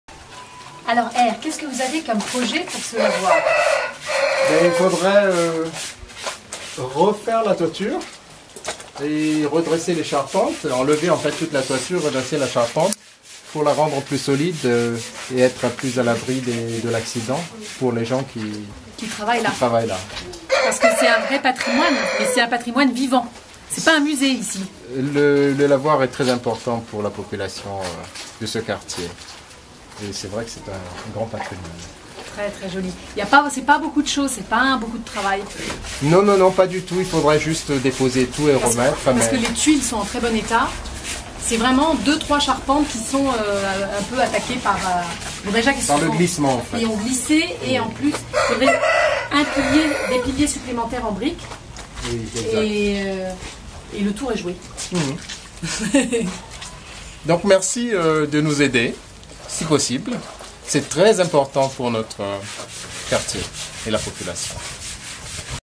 PROJET DE RESTAURATION DU LAVOIR DE TANANA AMBONY A FIANARANTSOA
Heritsialona, président de l'association de conservation de la haute ville de Fianarantsoa, nous présente ce projet simple et concret au bénéfice des habitants de ce quartier protégé.